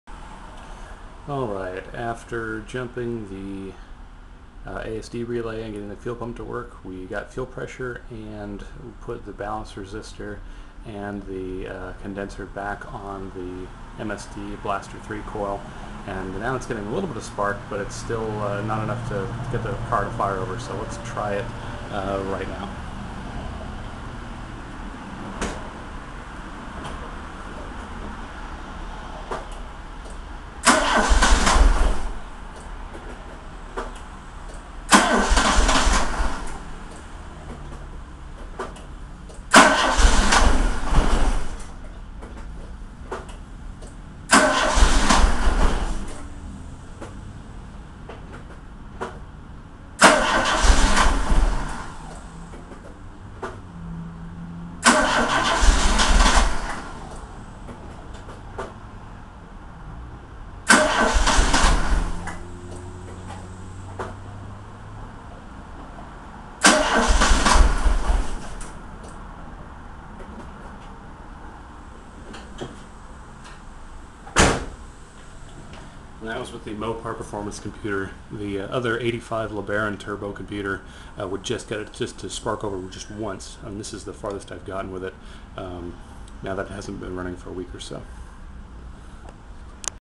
Dodge Omni GLH-T trying to start 6/20/2009
Spare Power Module fixed the no-fuel issue (ASD Relay bad likely), put the .8 ohm ballast resistor and condenser back on the MSD Blaster 3 coil. Car would now spark, but not start-- maybe flooded? '85 Lebaron Turbo LM and it would only turn over once or twice. put the MOPAR Performance LM in (unknown part # or stage of tune), and it would turn over much more aggressively. this video is with the MOPAR LM and spare PM. Any suggestions? Just let it sit in case it's flooded?